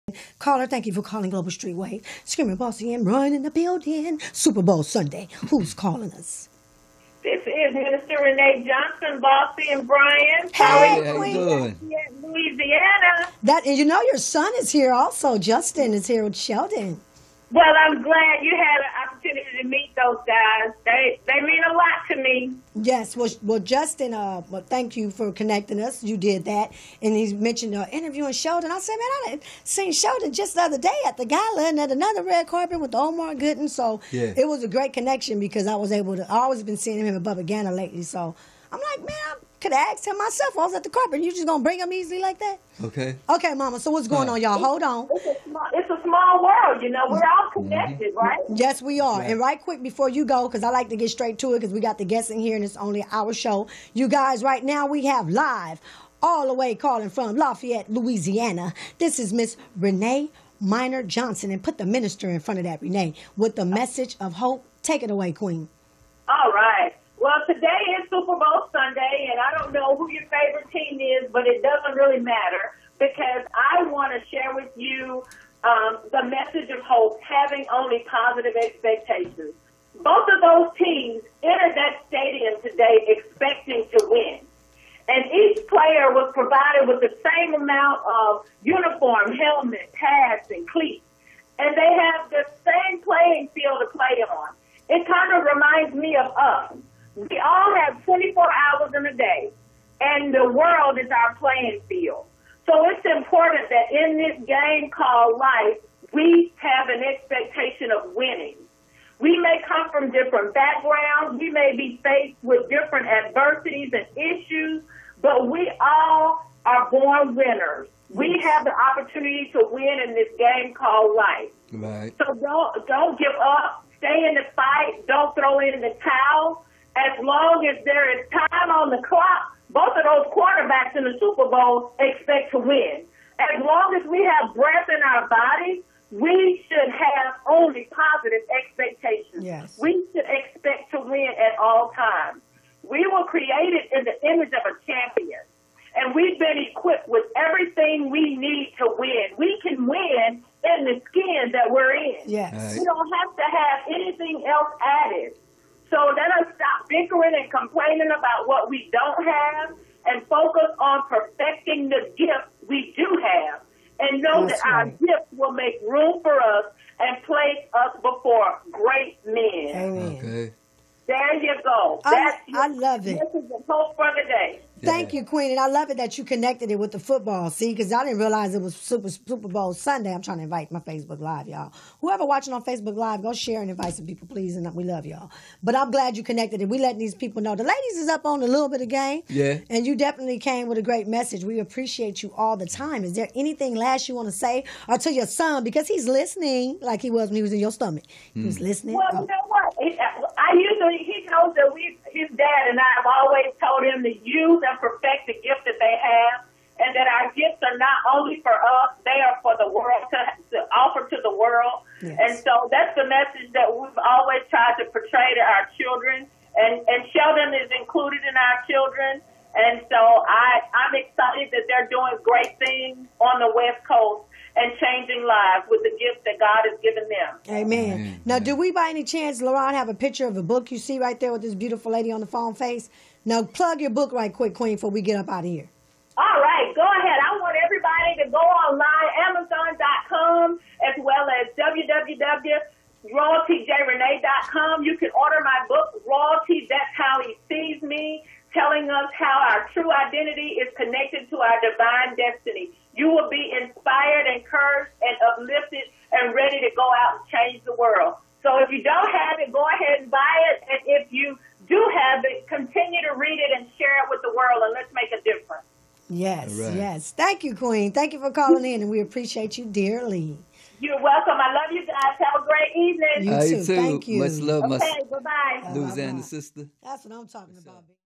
GLOBAL STREET WAVE - MESSAGE OF HOPE WITH MINISTER RENEE MINOR JOHNSON
At Morris Media Studios February 4, 2018 Superbowl Sunday